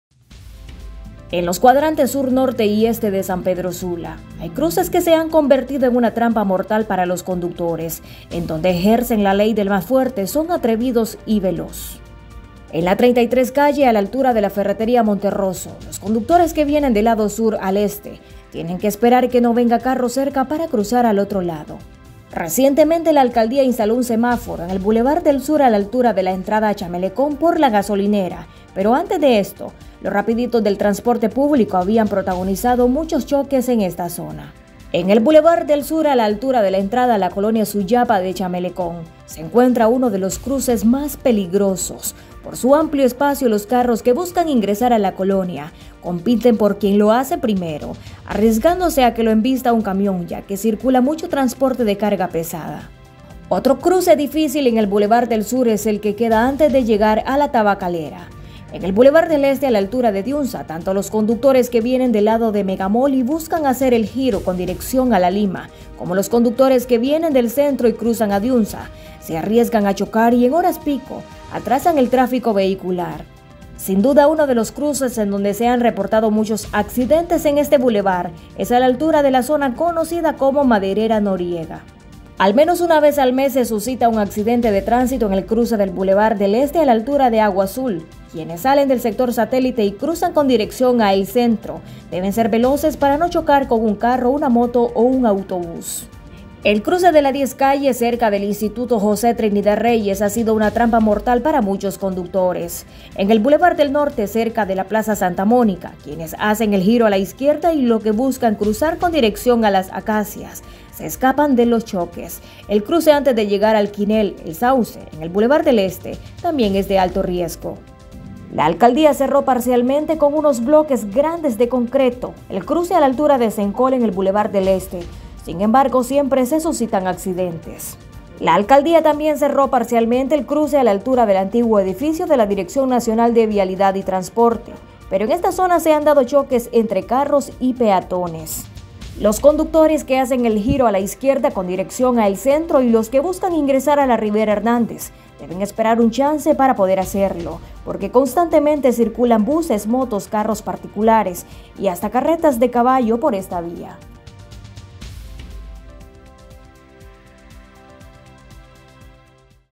Los cruces más peligrosos de San Pedro Sula
En los cuadrantes sur, norte y este de San Pedro Sula, hay cruces que se han convertido en una trampa mortal para los conductores, en donde ejercen la ley del más fuerte, son atrevidos y veloz.